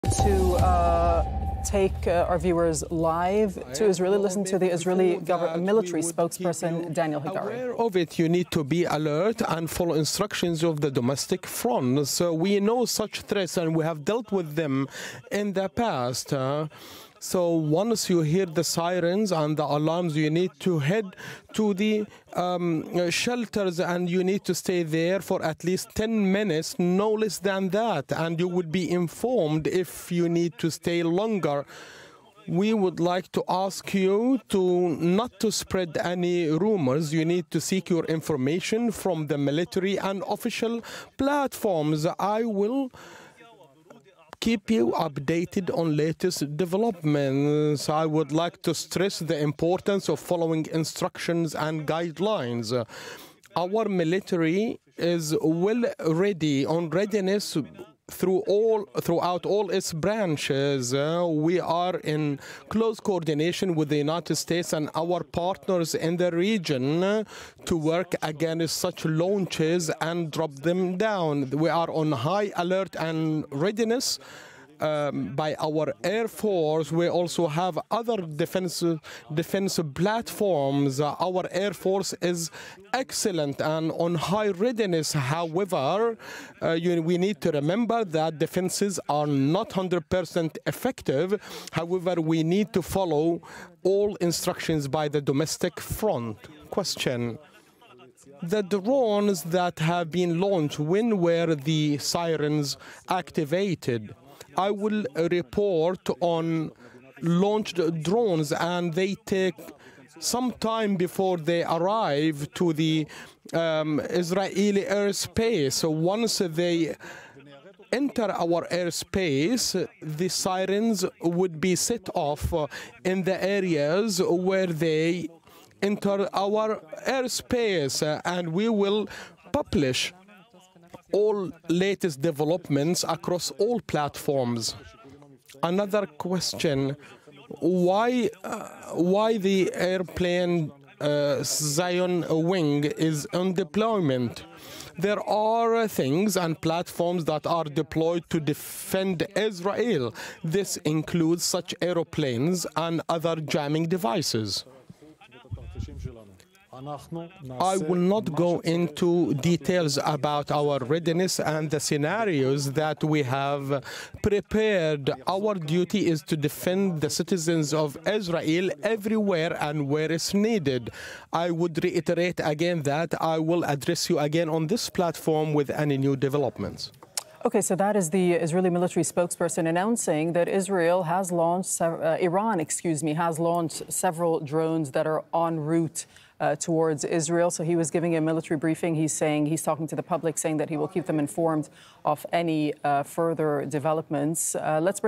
Iran has launched drones at Israel, will take hours to arrive: Israeli military
The Israeli forces say that sirens will sound in any areas that are threatened by the drones.